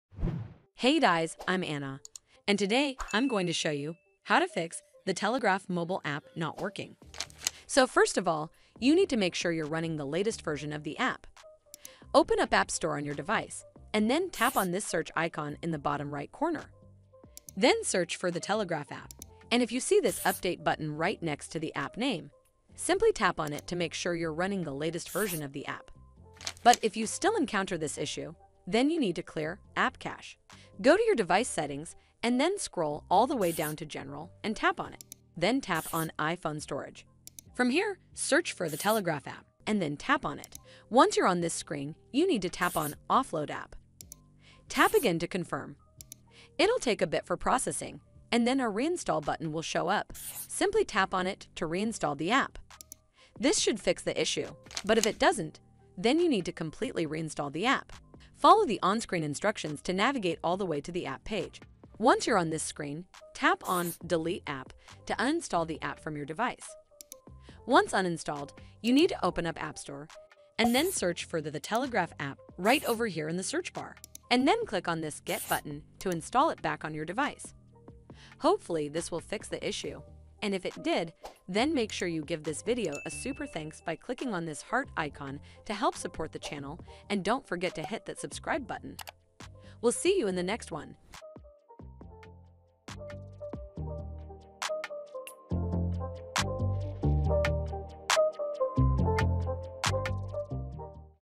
The Telegraph App Not Working: How to Fix The Telegraph App Not Working
In this video, I'll show you How to Fix The Telegraph App Not Working. This is the easiest and fastest way to Fix The Telegraph App Not Working. Make sure you watch until the end of this video to find out How to Fix The Telegraph App Not Working on ios. These methods work on iOS 11 as well as iOS 12, iOS 13, iOS 14, iOS 15 and iOS 16. Hope you enjoy!

Video Parts:
00:00 Intro: How to Fix The Telegraph App Not Working
00:07 1.Solution: Update The Telegraph App
00:27 2.Solution: Clear The Telegraph App cache files
00:57 3.Solution: Delete and Reinstall The Telegraph App
01:19 Outro: Ending